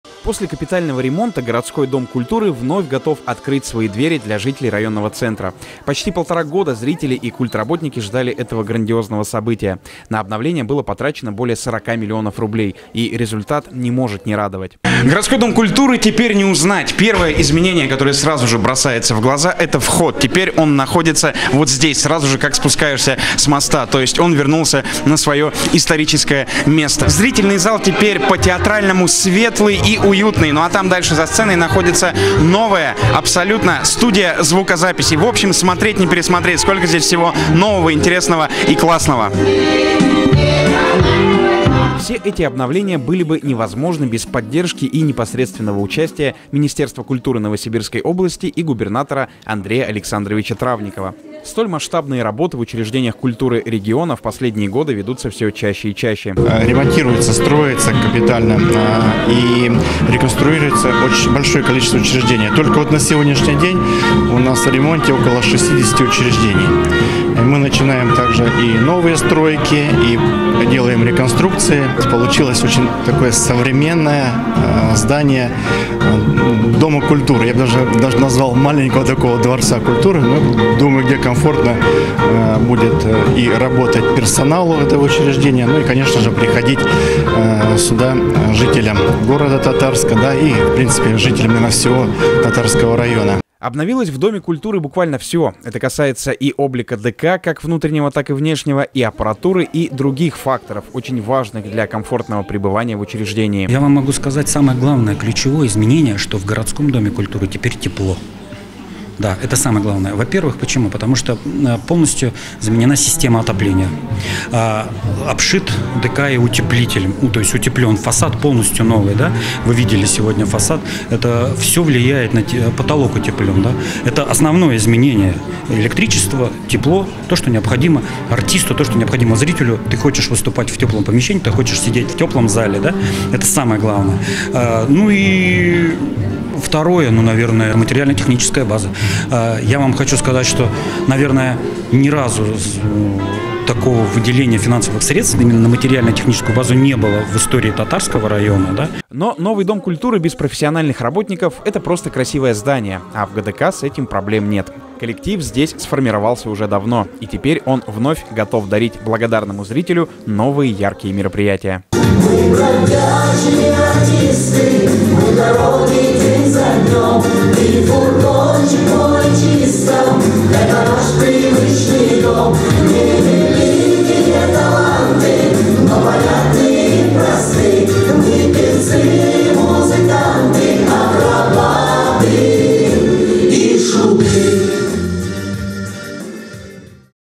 Городской Дом культуры официально распахнул двери после ремонта
После капитального ремонта Городской Дом культуры вновь готов открыть свои двери для жителей районного центра. Почти полтора года зрители и культработники ждали этого грандиозного события.